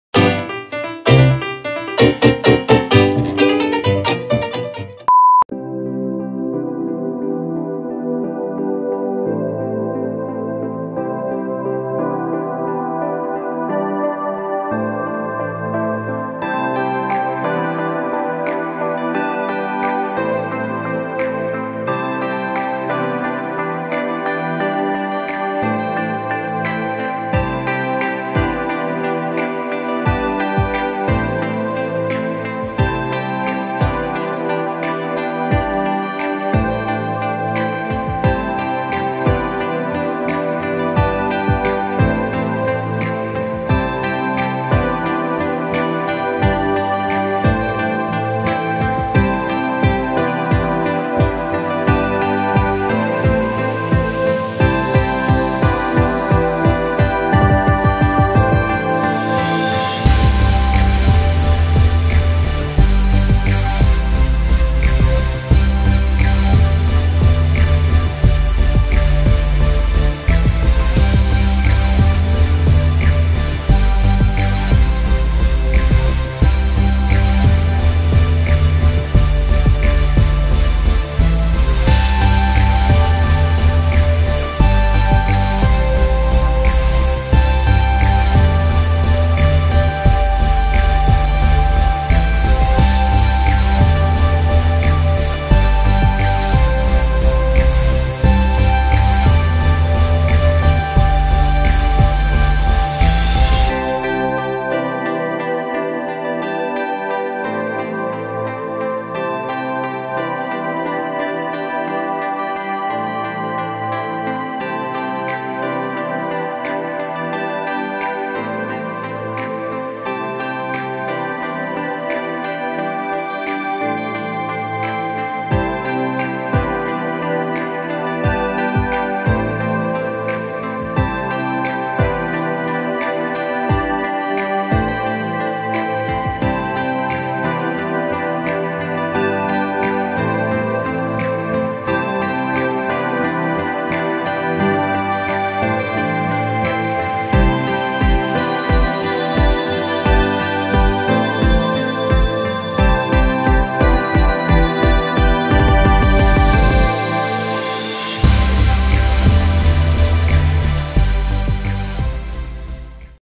Meet Apo Whang-Od, the Last Kalinga Tattoo Artist plus Sagada tour.
See you on the next travel